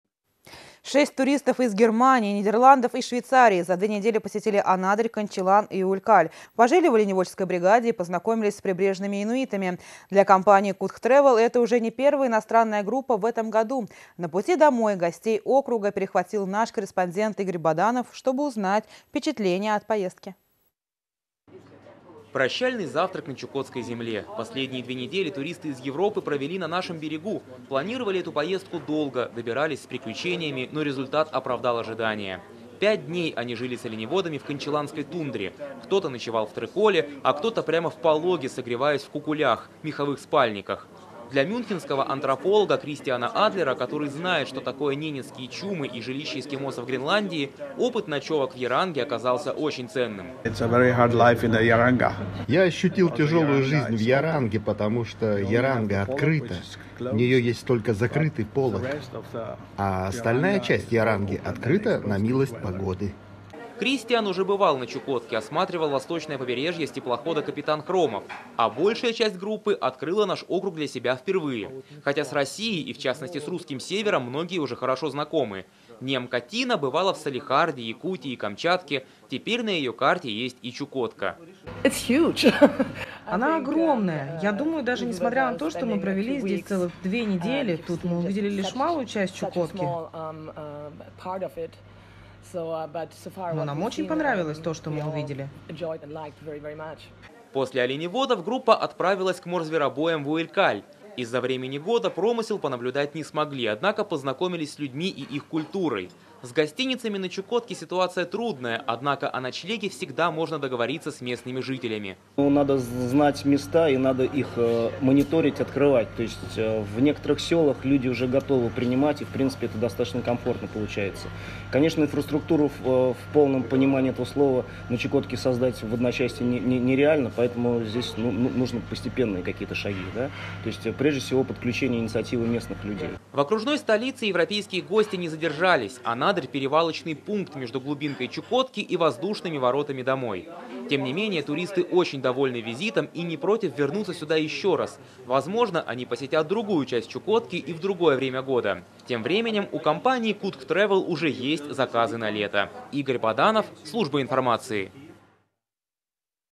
Интуристы
Шесть туристов из Германии, Нидерландов и Швейцарии за две недели посетили Анадырь Канчалан и Уэлькаль. Пожили в оленеводческой бригаде и познакомились с прибрежными инуитами. Для компании Кутх Трэвел это уже не первая иностранная группа в этом году. На пути домой гостей округа перехватил наш корреспондент Игорь Баданов чтобы узнать о впечатлениях от поездки.